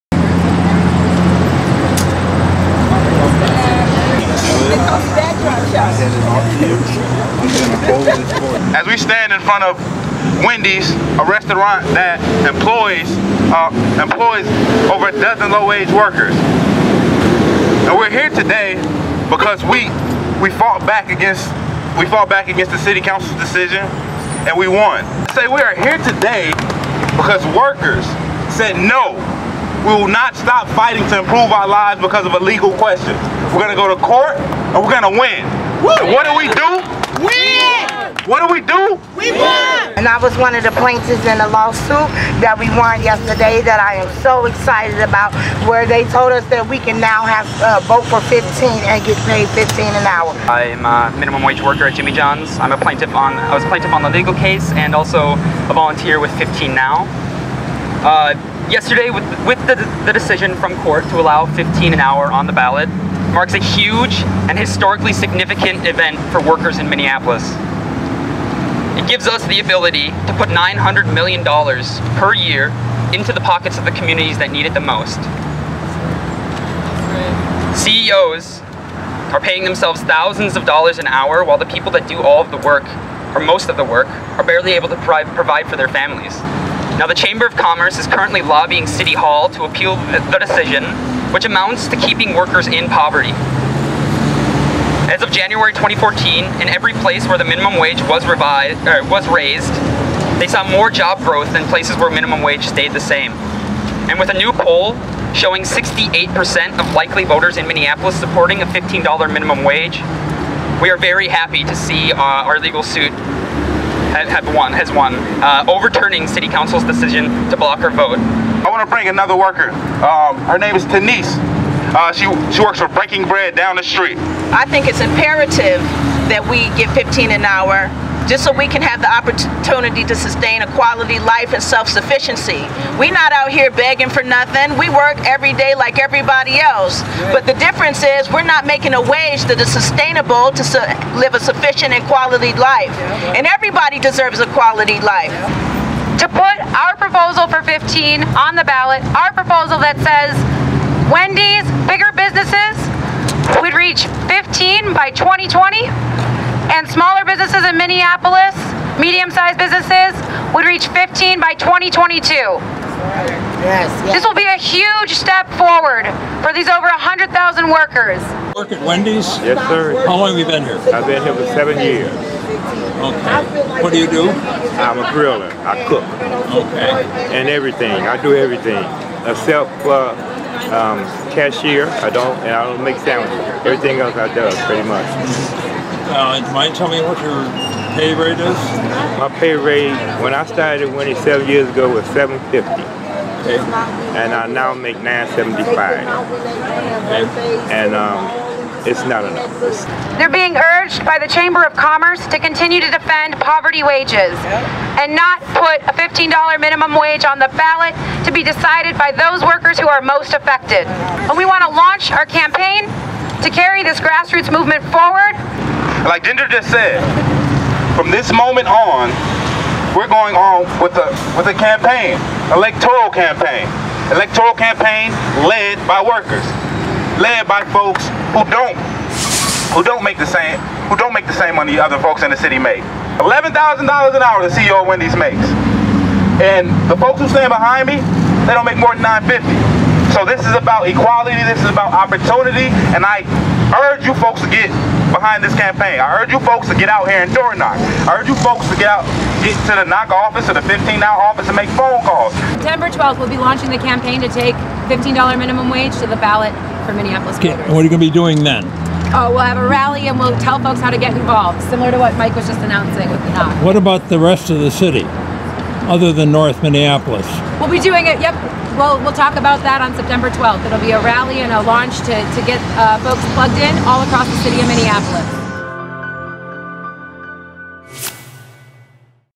Minneapolis Minimum Wage Campaign Kicks Off
Supporters of a higher minimum wage are are focused on winning votes in Minneapolis, while the city's attorney is racing the clock to stop the issue from going on the ballot.

Fresh off a court victory saying the $15 an hour minimum wage is an appropriate topic for a charter amendment, 15Now laid out the next steps in its campaign on Tuesday. Leaders of the group addressed the media outside of a Wendy's restaurant that would need to raise its wages if the amendment passes.

"$11,000 an hour the CEO of Wendy's makes," said Rod Adams of Neighborhoods Organizing for Change (NOC). "And the folks standing behind me, they don't make more than $9.50. So this is about equality. This is about opportunity. And I urge you folks to get behind this campaign."

"I think it's imperative that we get $15 an hour just so we can have the opportunity to sustain a quality life and self-sufficiency," said Tanise Hodges of 15Now. "We work every day like everybody else. But the difference is we're not making a wage that is sustainable to live a sufficient quality life. And everybody deserves a quality life."

If approved by voters, minimum wages in Minneapolis would move to $10 an hour in 2017 and reach $15 an hour in 2020. For businesses with less than 500 employees, the minimum wage wouldn't hit $15 an hour until 2022.

15Now, NOC and Centro de Trabajadores Unidos en Lucha (CTUL) plan to launch a citywide campaign on September 12 to get voters on their side. However, Minneapolis City Attorney Susan Segal said her office had filed a motion in Hennepin County District Court to stay the judge's order that put the minimum wage on the ballot.

The city is also asking for an accelerated review by the Minnesota Supreme Court, which may be needed because the deadline for finalizing the ballot is this Friday.

In an emailed statement, Segal said, "We remain of the strong conviction that the petition to place a minimum wage ordinance on the ballot for inclusion in the city's charter is in conflict with Minnesota statutes and the Minneapolis Charter which contains no provision allowing such initiatives."